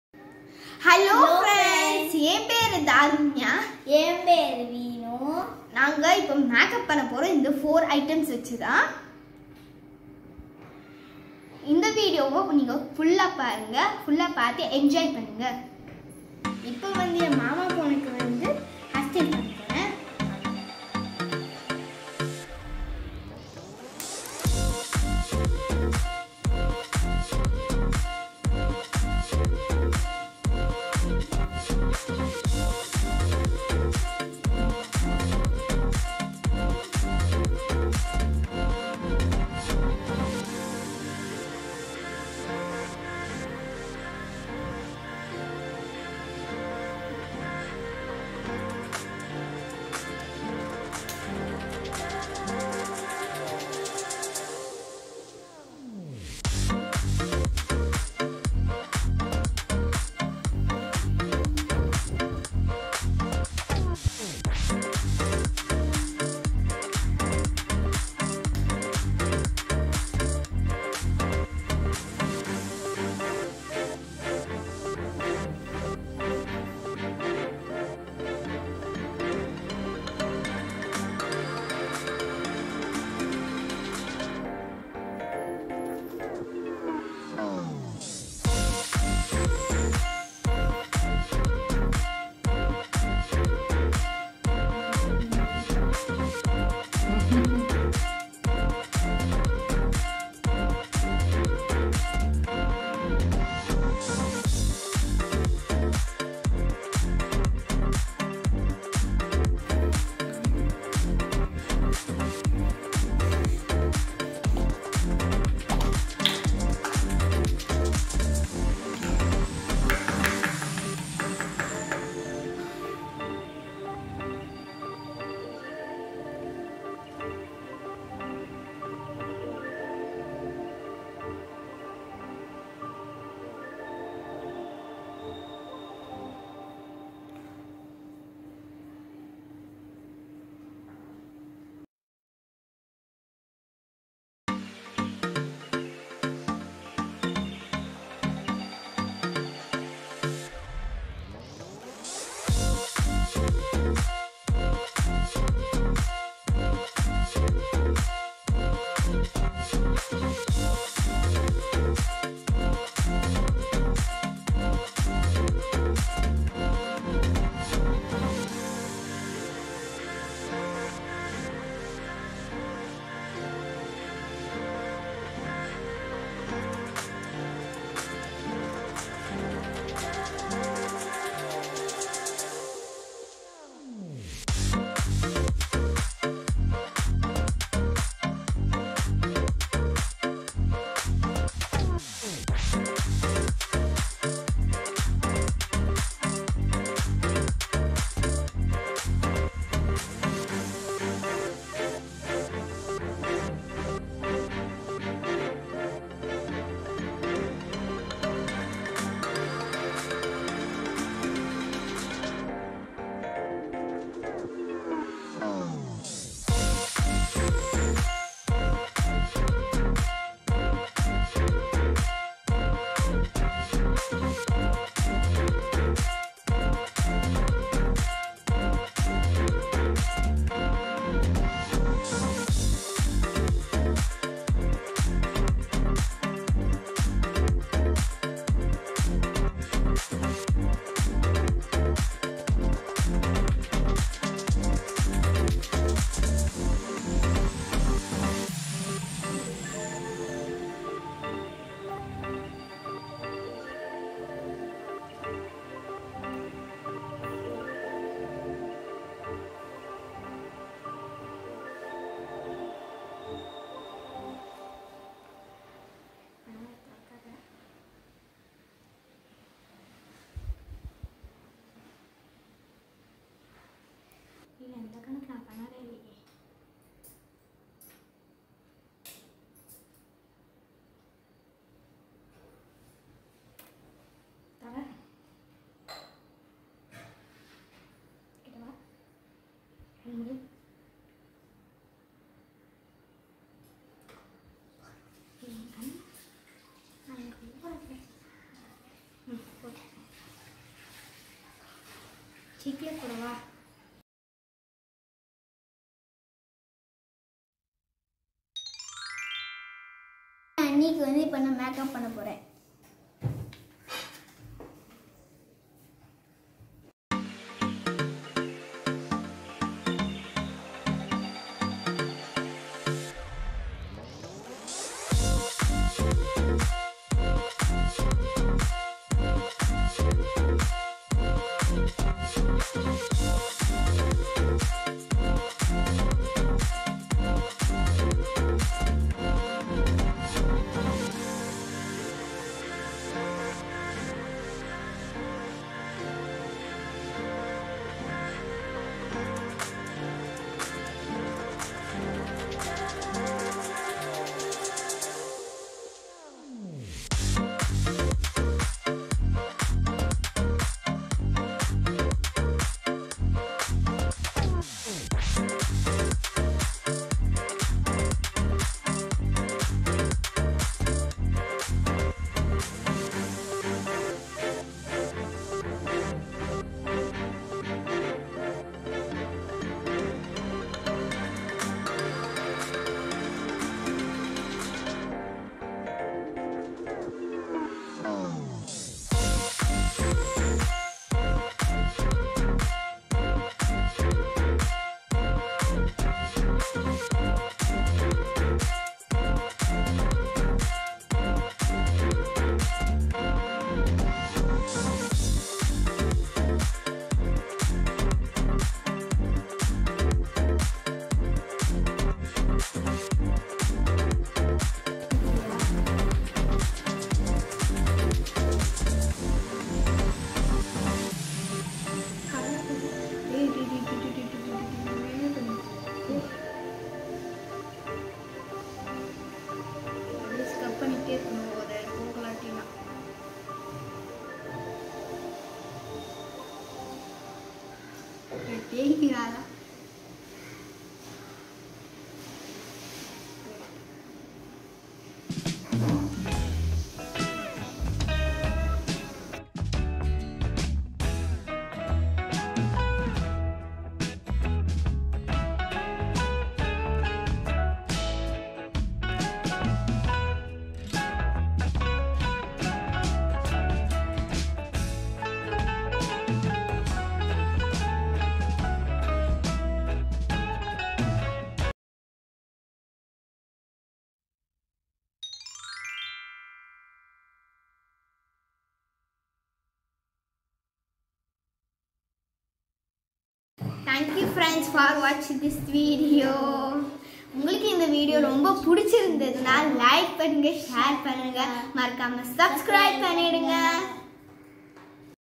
Kids Makeup || Simple look || Tamil
Fun with the makeup looks. learning the makeup. Kids enjoying the work.